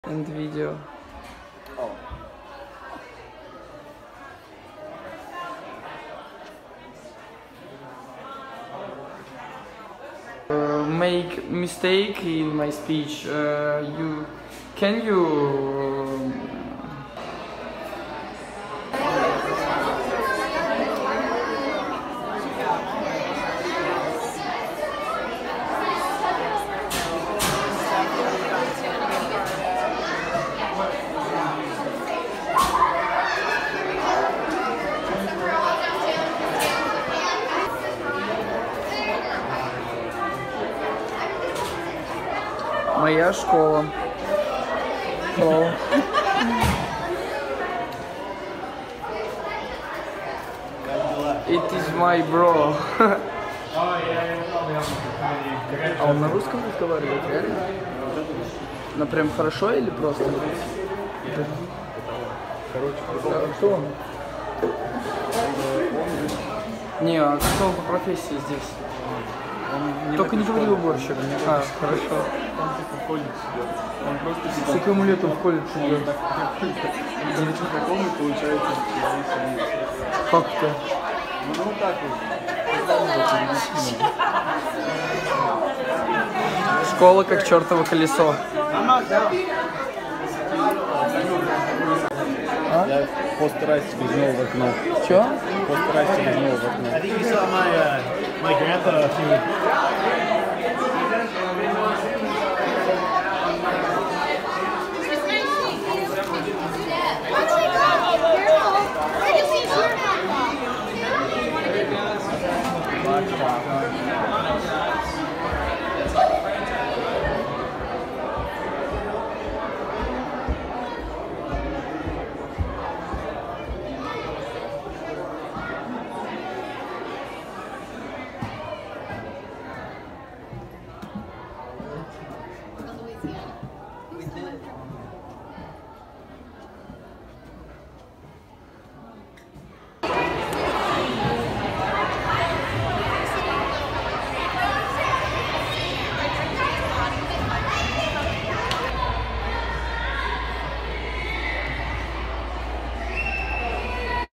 Inside an American Christian School
So I came to my new American school in Montana for the first time